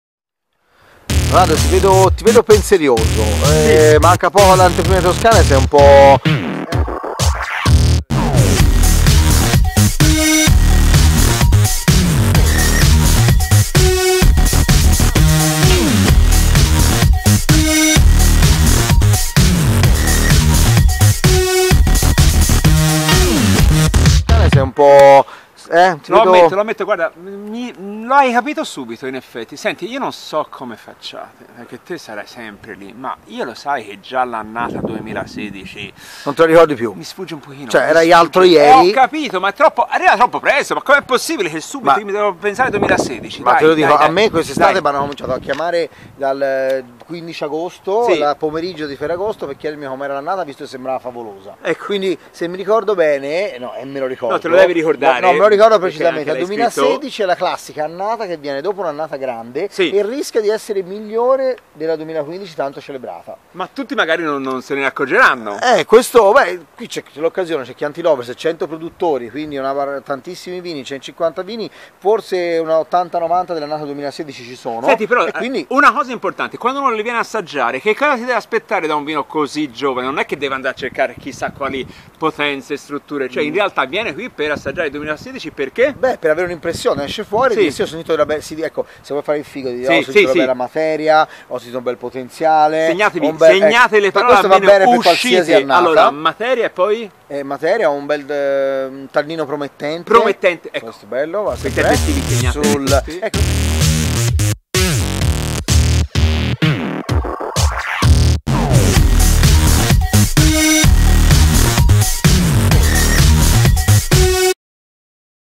chiantilovers2017 Chianti annata 2016 in esame
Impariamo a parlare come un vero degustatore da qui a domenica, torna Chiantilovers!